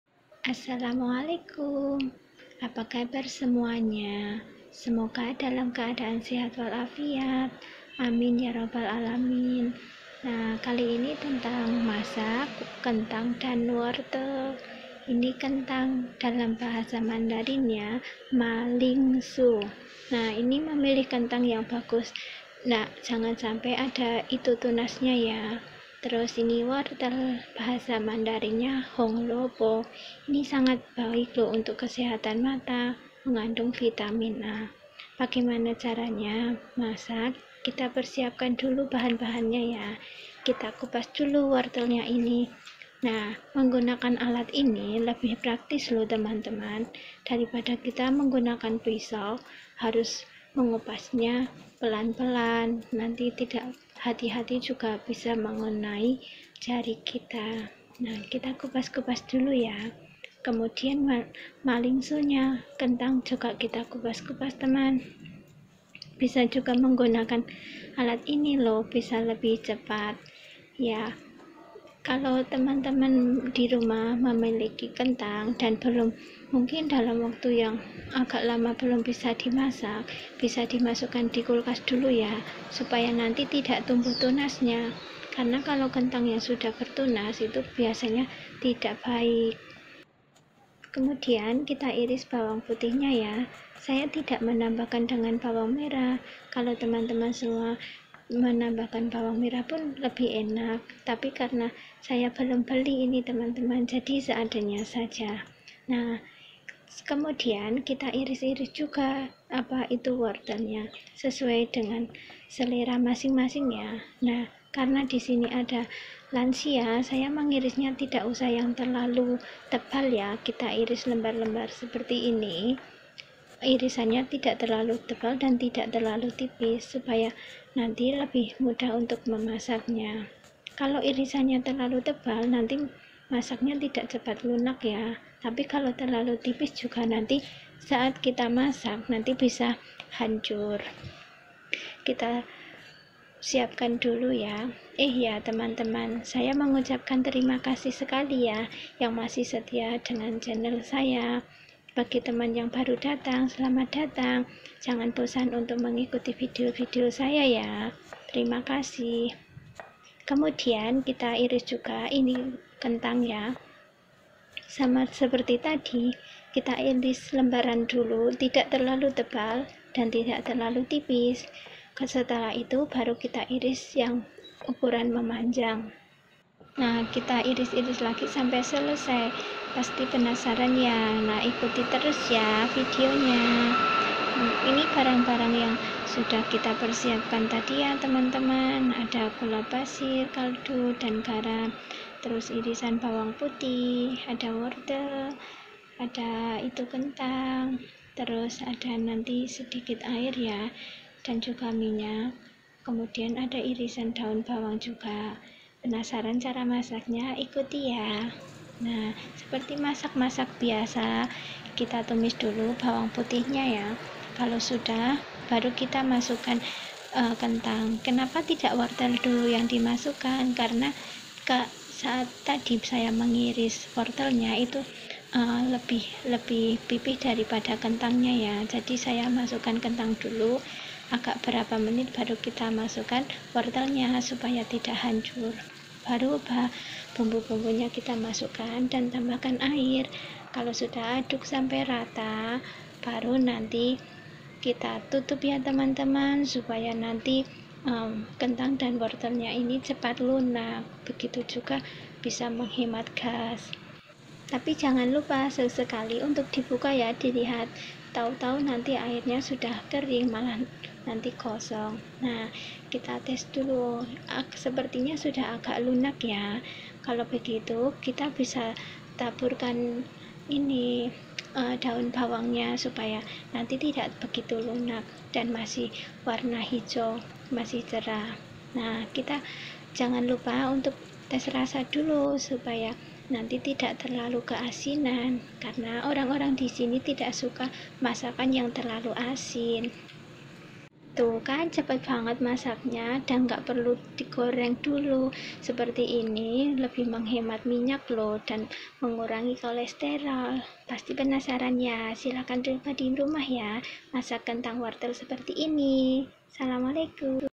resep tumis kentang wortel tanpa digoreng dulu pun enaknya kebangetan
Resep tumis kentang dan wortel tanpa digoreng dulu rasanya enak banget.
Kentang dan wortel dimasak seperti ini bisa menghemat minyak dan mengurangi kolesterol.
Bahan:
1 kentang
sedikit wortel
2siung bawang putih
gula pasir,garam,kaldu
daun bawang
minyak dan air
Cara masak dan persiapan bah as n ada di video ya.
Terimakasih atas support dan kunjunganya  🙏🙏🙏